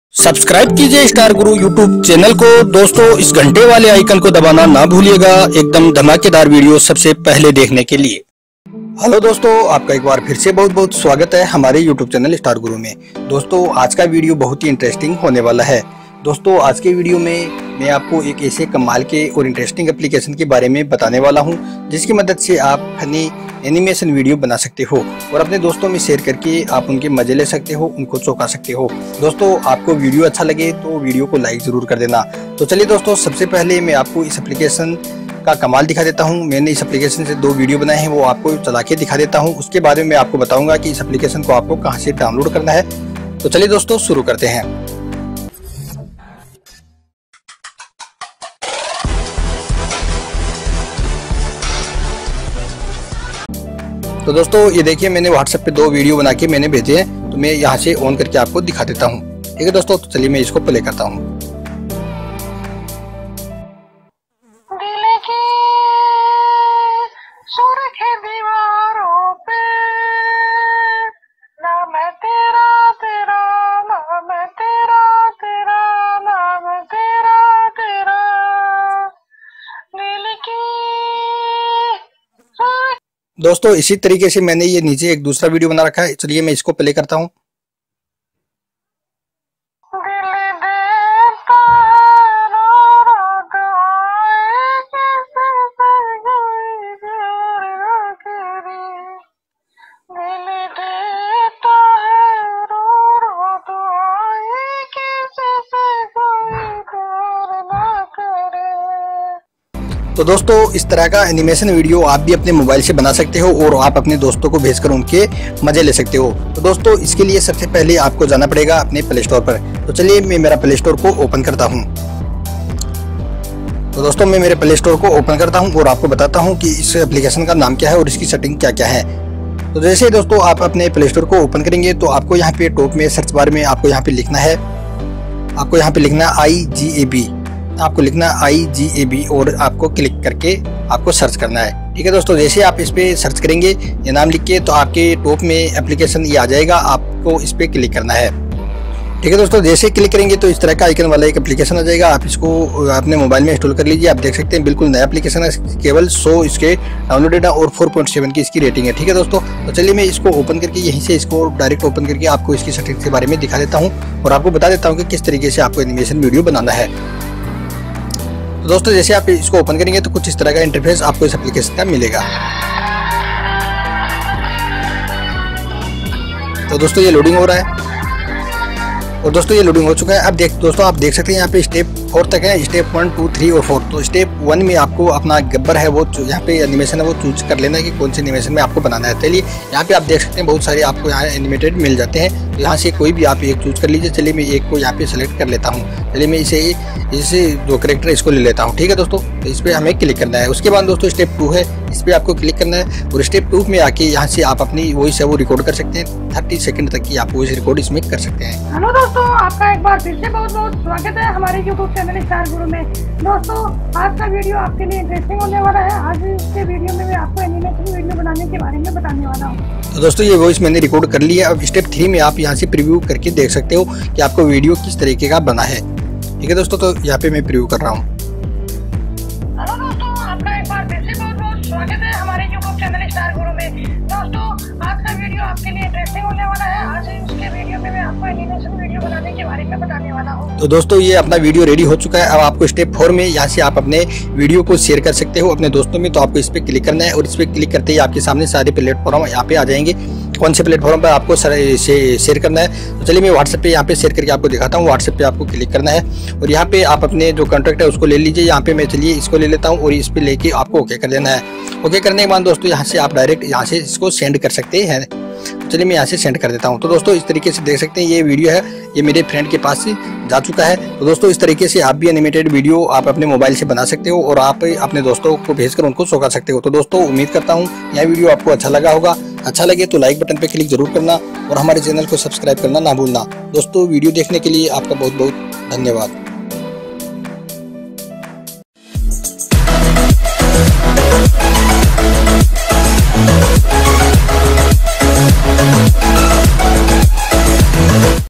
फनी वीडियो बनाओ / हंसाने वाले वीडियो बनाओ / Best New Funny Video Maker App Hindi Urdu
दोस्तों आज के वीडियो में आपको एक ऐसे अप्प के बारे में बताया गया है जिसकी मदद से आप अपने मोबाइल से फनी वीडियो बना सकते हो , हंसाने वाले वीडियो बना सकते हो दोस्तों बहुत ही इंटरस्टिंग अप्प है इस अप्प से वीडियो बनाकर आप अपने दोस्तों के मजे ले सकते हो तो दोस्तों देखते रहिये विडियो और हां दोस्तों अगर आपको विडियो पसंद आये तो प्लीज लाइक और कमेंट जरूर करिये और अगर आपको किसी टॉपिक पे विडियो बनवाना है तो मुझे कमेंट बॉक्स में जरूर लिखिए